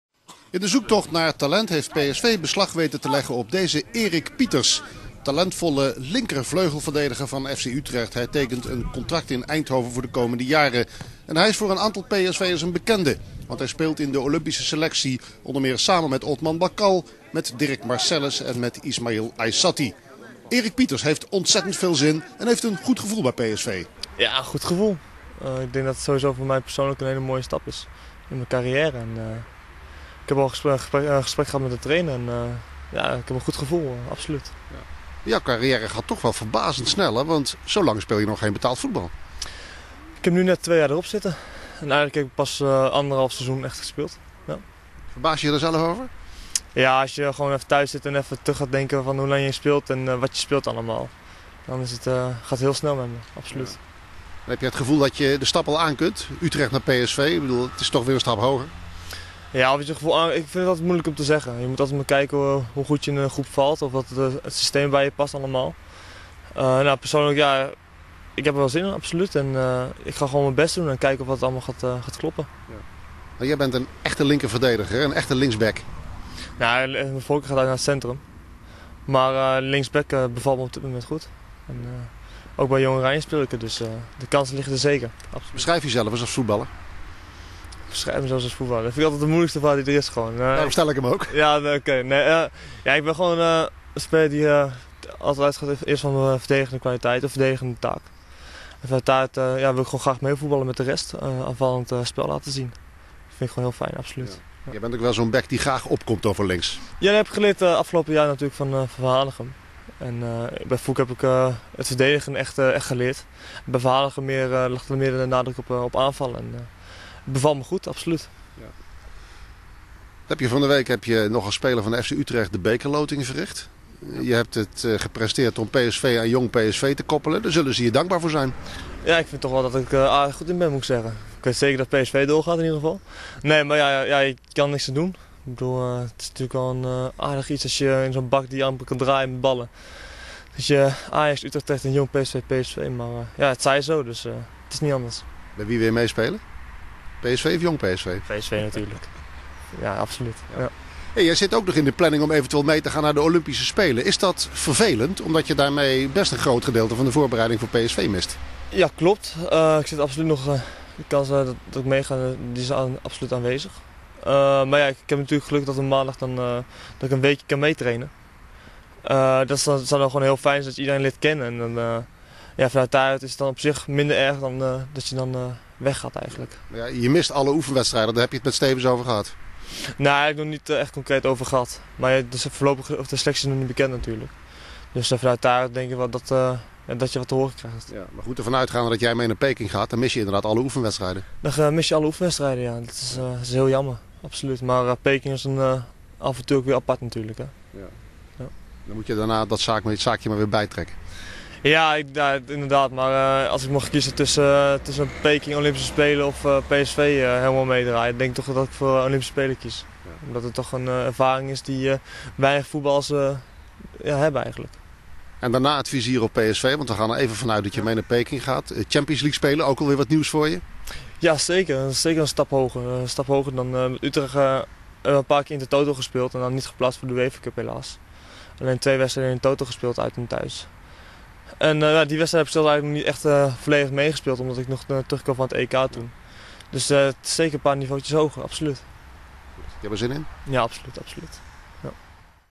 PSV strikt talentvolle linksback Erik Pieters (19)
PSV heeft Erik Pieters overgenomen van FC Utrecht. De talentvolle linksback tekent in Eindhoven een contract voor vier jaar. Pieters is een belangrijke kandidaat om met Jong Oranje mee te gaan naar de Olympische Spelen in Peking.